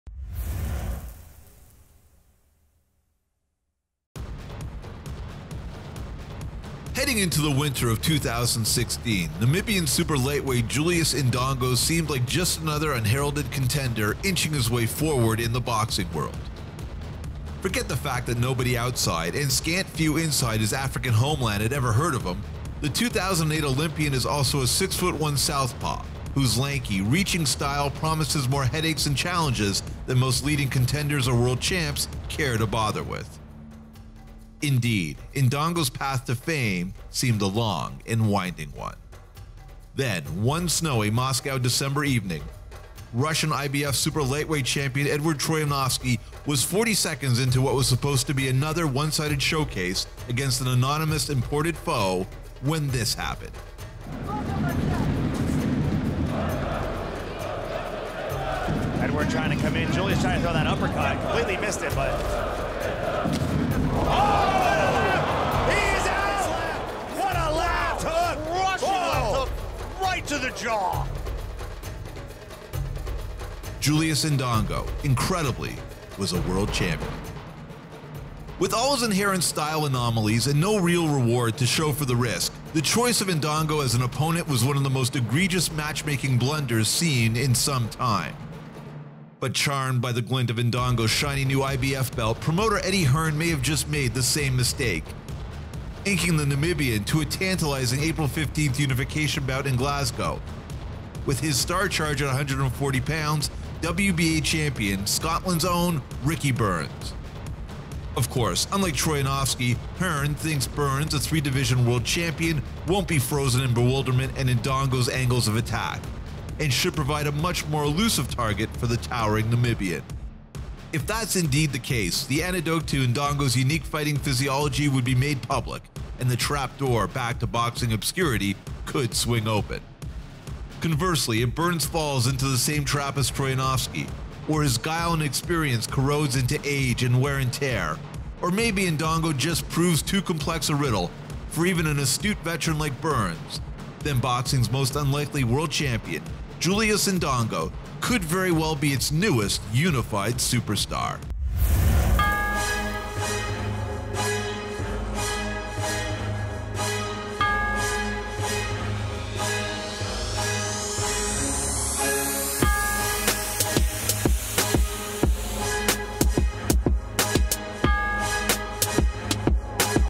Ricky Burns vs. Julius Indongo Preview
This weekend, Glasgow, Scotland will host an epic super lightweight showdown between WBA kingpin Ricky Burns and new IBF champ Julius Indongo.